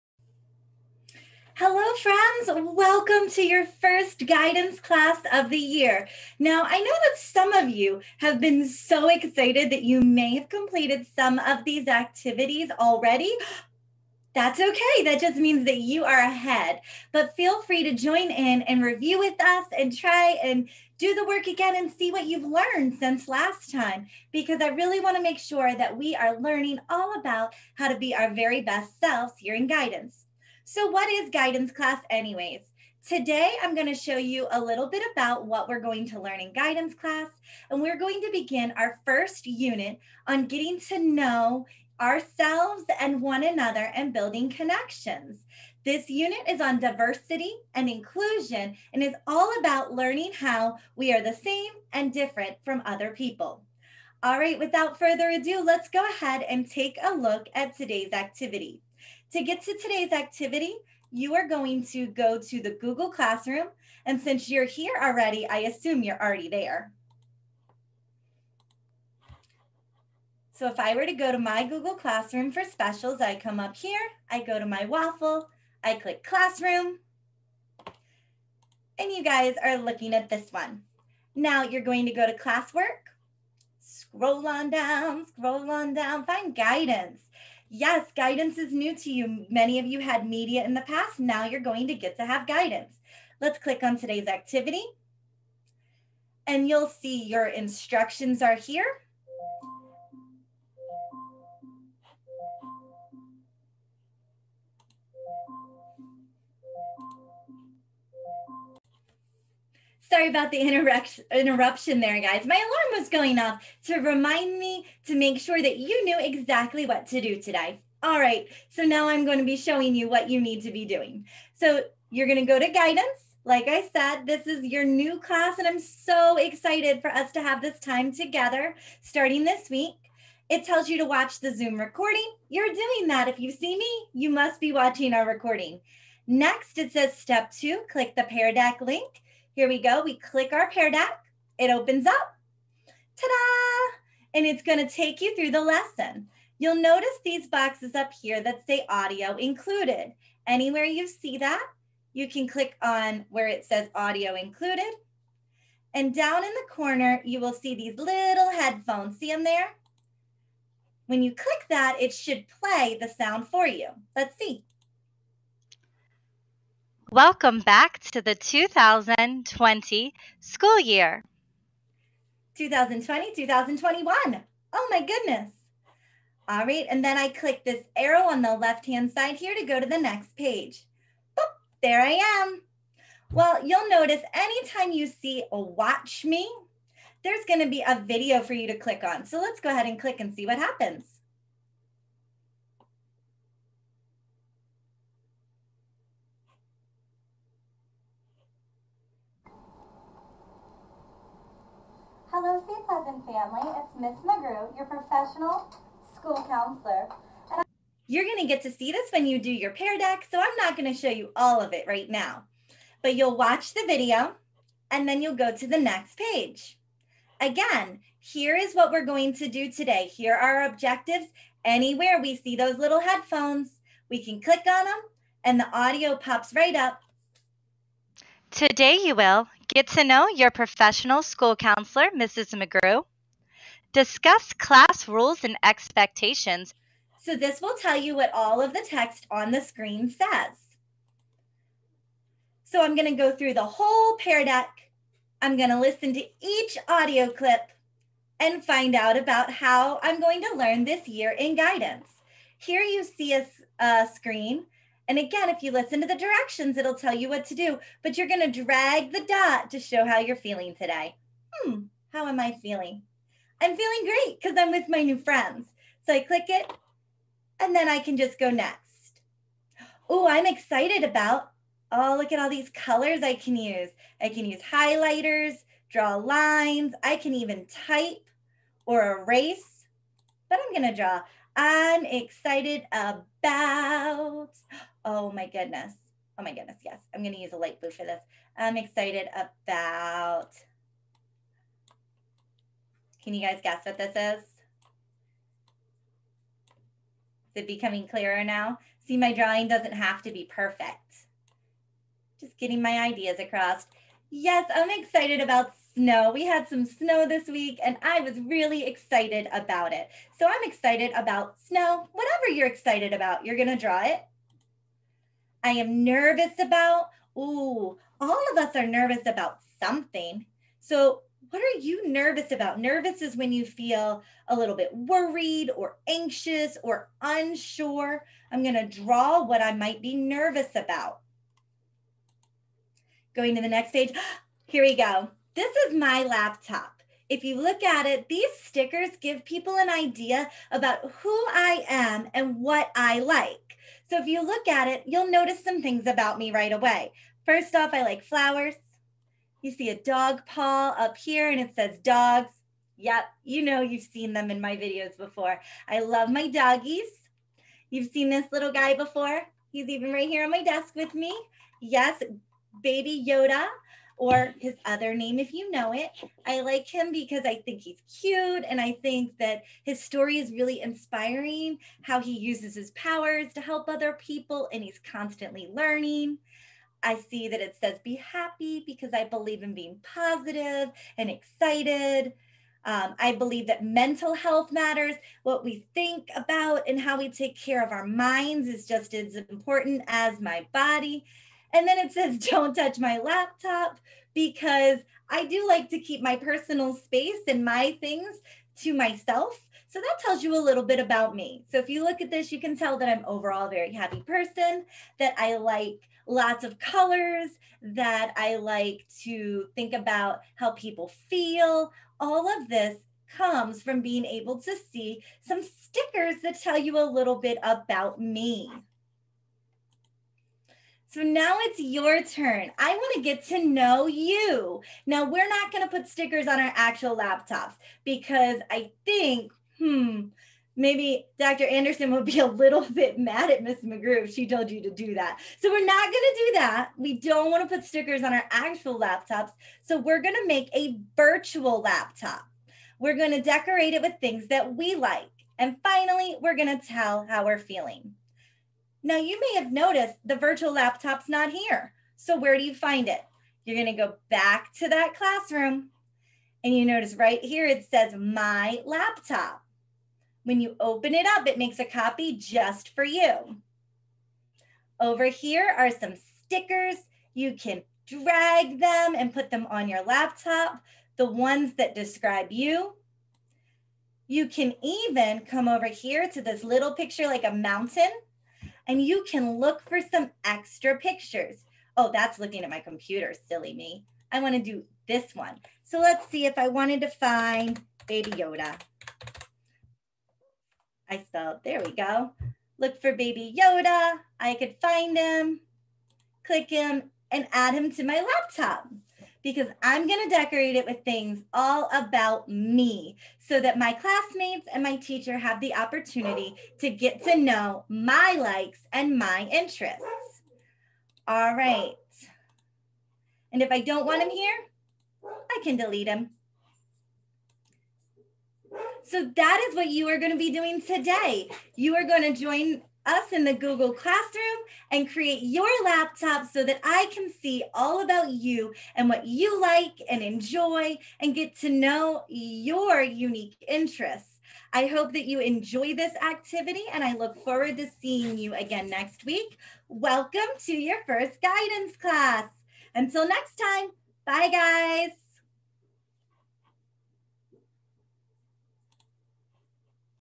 Guidance Intro Lesson (My Laptop)
This is My Unit 1 Lesson 1 activity for my school counseling program. It helps students to get to know one another and express themselves during virtual learning.